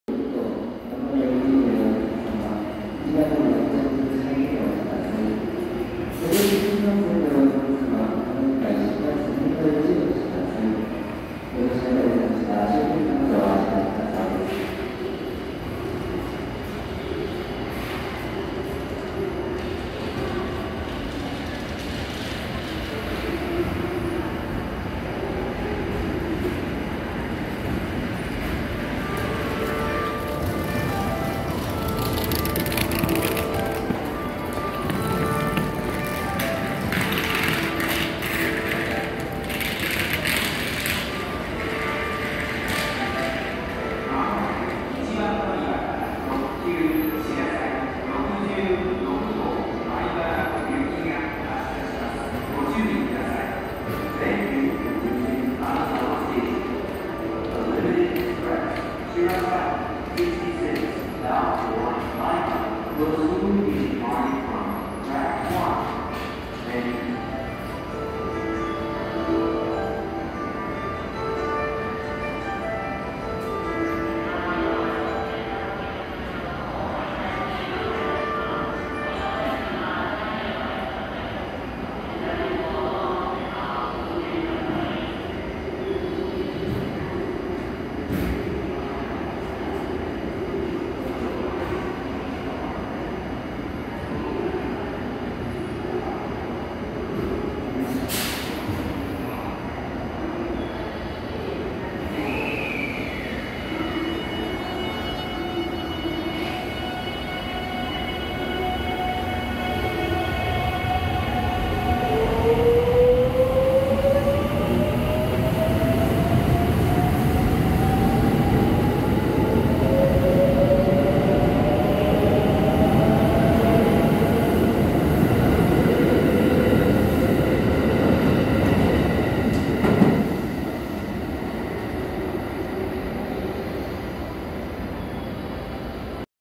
【まもなく見納め】681系0番台 特急しらさぎ66号 金沢駅出発シーン
撮影地、金沢駅1番線ホーム
列車名称、特急しらさぎ66号米原行き
発車時刻、金沢駅20:06発
列車形式、681系0番台(6両編成)

2024年3月16日のダイヤ改正で、北陸新幹線金沢～敦賀間延伸に伴い、特急列車が全て敦賀止まりに短縮されます。そのため、金沢駅での特急列車が見納めとなりますので、撮影して来ました。